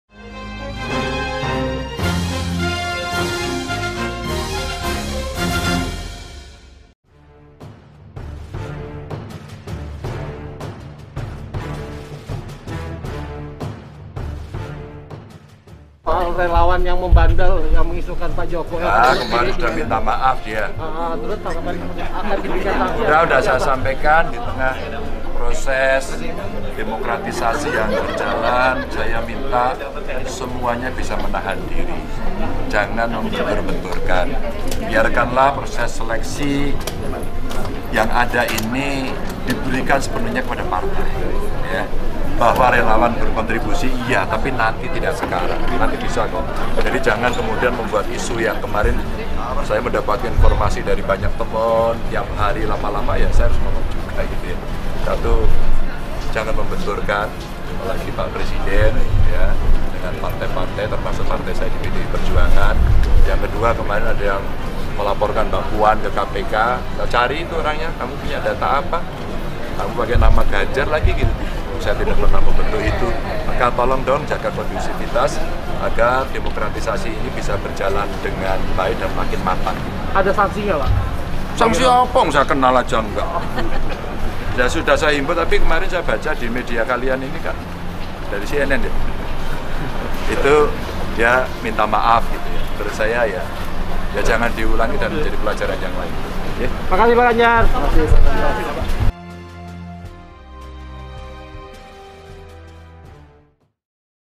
GANJAR TANGGAPI RELAWAN YANG LAPORKAN PUAN KE KPK
Gubernur Jawa Tengah, Ganjar Pranowo meminta semua pihak menahan diri. Tidak boleh lagi, ada pihak apalagi yang mengatasnamakan relawan Ganjar yang membentur-benturkan satu pihak dengan pihak lain atau membenturkan partai politik.

Hal itu disampaikan Ganjar terkait adanya relawan yang mengatasnamakan dirinya yang melaporkan Puan Maharani ke KPK, baru-baru ini. Ada juga pihak yang mengatasnamakan relawan Ganjar yang membenturkan Presiden Joko Widodo dengan PDIP.

"Sudah, kemari pihak-pihak itu sudah minta maaf. Sudah saya sampaikan, di tengah proses demokratisasi yang berjalan saya minta semuanya menahan diri. Jangan membentur-benturkan," tegasnya saat ditemui usai memberikan orasi ilmiah Dies Natalis Universitas Pancasila Jakarta, Selasa (1/11).

Ganjar meminta semua pihak bersabar dan memberikan proses seleksi yang ada sepenuhnya pada partai.

"Bahwa relawan nanti akan berkontribusi, iya. Tapi nanti, bukan sekarang. Nanti bisa kok, jadi jangan buat isu yang nggak baik," tegasnya.

Ganjar menyesalkan apa yang terjadi saat ini. Awalnya ia mengaku hanya diam saja, namun karena banyaknya informasi yang diterima, akhirnya Ganjar buka suara.

"Karena tiap hari saya dapat informasi itu, lama-lama ya saya harus ngomong juga. Janganlah membentur-benturkan apalagi membenturkan pak Presiden Jokowi dengan partai termasuk dengan partai saya di PDIP. Kedua kemarin ada yang melaporkan mbak Puan ke KPK. Saya cari itu orangnya," tegasnya.

Ganjar mengatakan mencari relawan yang melaporkan Puan ke KPK. Ia menanyakan apakah dia punya data valid atau hanya menyebar hoax.

"Ini kan bahaya, apalagi dia pakai nama Ganjar lagi. Wong saya tidak pernah membentuk itu (relawan). Maka tolong jaga kondusivitas agar demokratisasi berjalan makin baik dan matang," tegasnya.

Ganjar lega karena relawan-relawan yang mengatasnamakan Ganjar dan melakukan manuver itu telah minta maaf. Ditanya apakah Ganjar memberikan sanksi, Ganjar tertawa dan mengaku tidak mengenal mereka.

"Sanksi apa, lha wong saya kenal saja tidak. Cuma saya baca di media katanya mereka sudah minta maaf. Menurut saya ya jangan diulangi dan jadi pembelajaran untuk yang lain," pungkasnya.